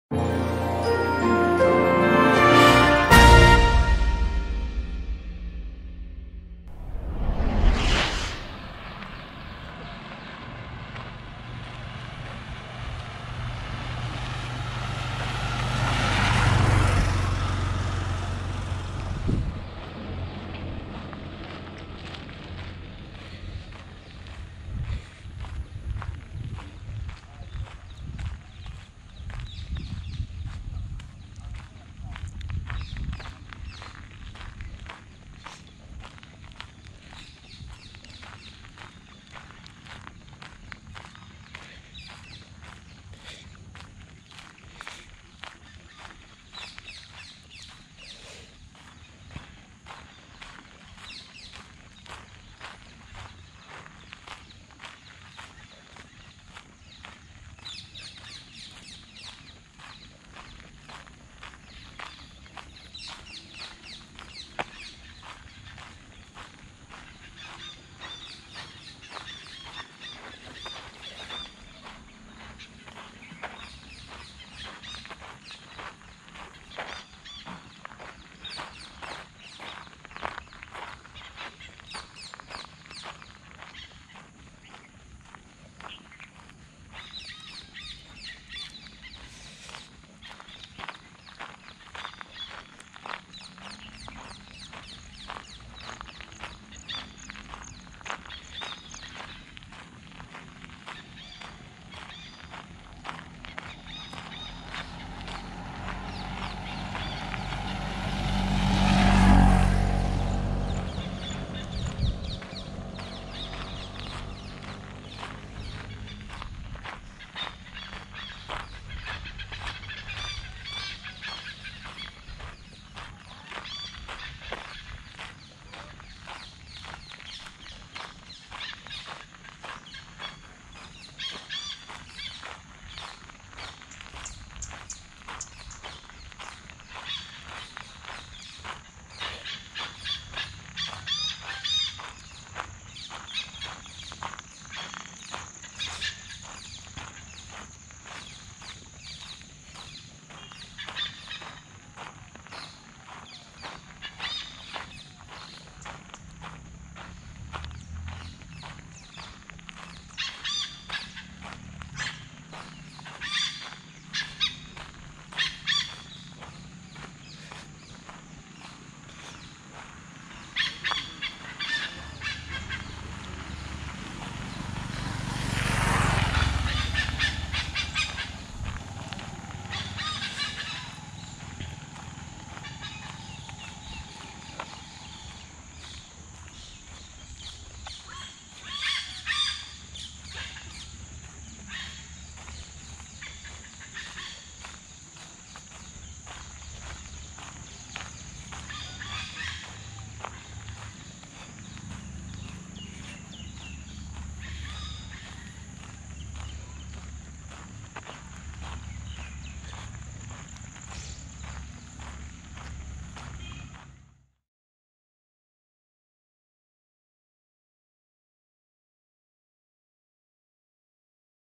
Relaxing Walk in a Rural and Scenic Sri Lankan Village
Experience a relaxing walk in a typical Sri Lankan village while listening to the sounds of trees and other ambient sounds. This particular clip was captured from a village called Nikawewa in Kurunegala district.